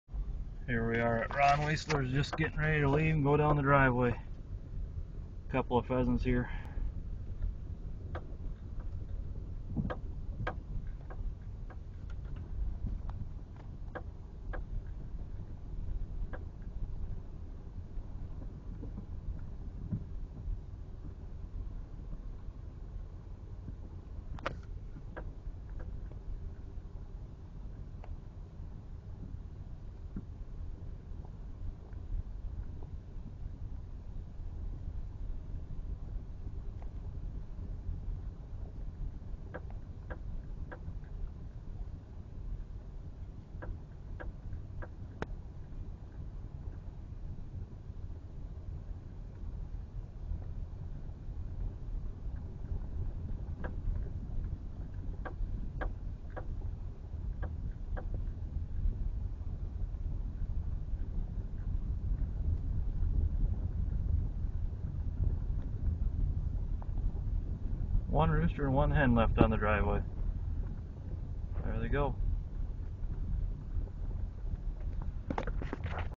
A few pheasants near Polo, SD
Just leaving a farm near Polo, SD and had to go slow so I didn't take out any pheasants. Video recorded on my Blackberry Bold 9650.